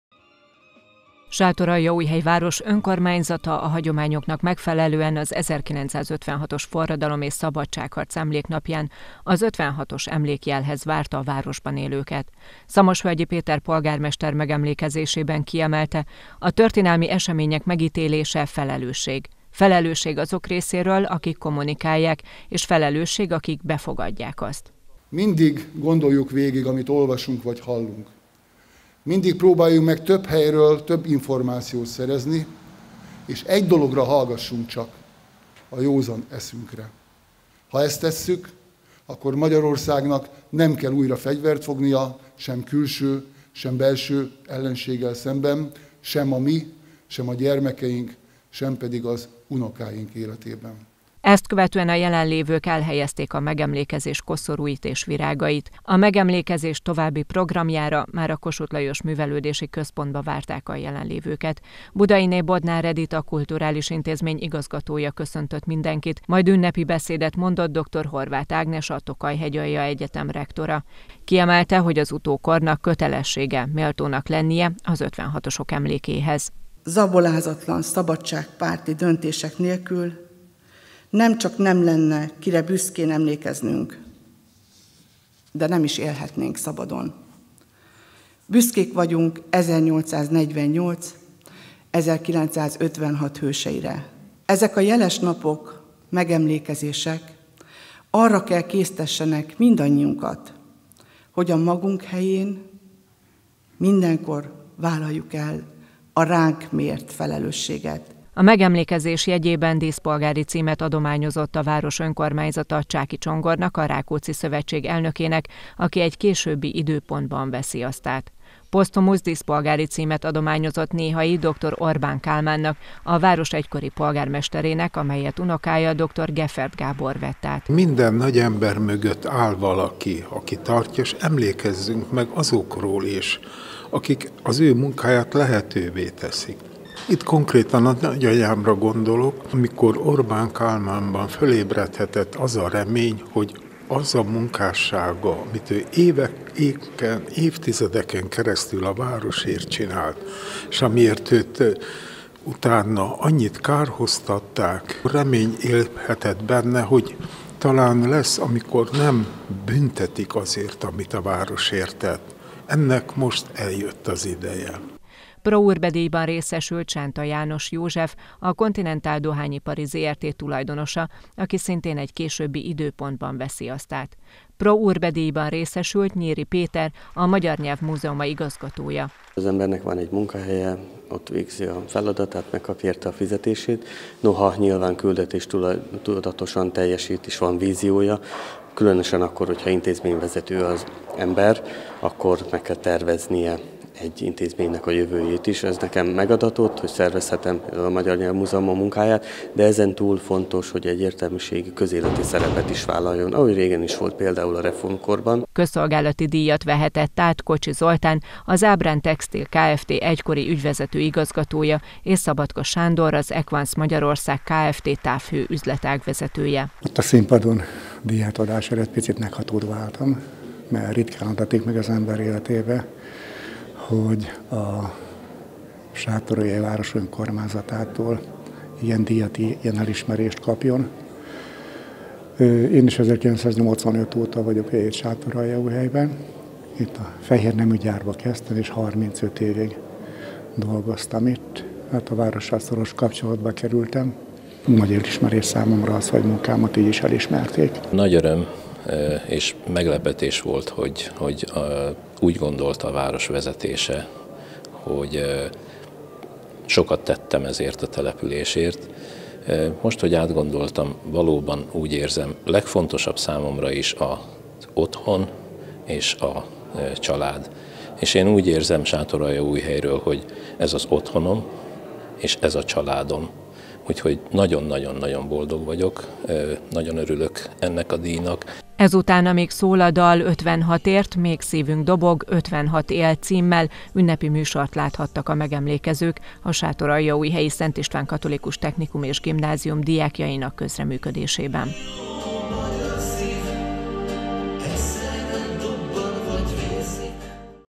Megemlékezésre várták a sátoraljaújhelyieket az 1956-os forradalom és szabadságharc napján
Sátoraljaújhely Város Önkormányzata és a Kossuth Lajos Művelődési Központ az 1956-os forradalom és szabadságharc 66. és a Magyar Köztársaság kikiáltásának 33. évfordulóján megemlékezésre várta a városban élőket. Ennek jegyében először az ’56-os emlékjelnél Szamosvölgyi Péter polgármester mondott emlékbeszédet. Majd a jelenlévők elhelyezték a kegyelet virágait. Ezt követően a Latabár Színházban a megemlékezés keretében beszédet mondott Dr. Horváth Ágnes, a Tokaj-Hegyalja Egyetem rektora. Idén is díjakat adták át az arra érdemeseknek.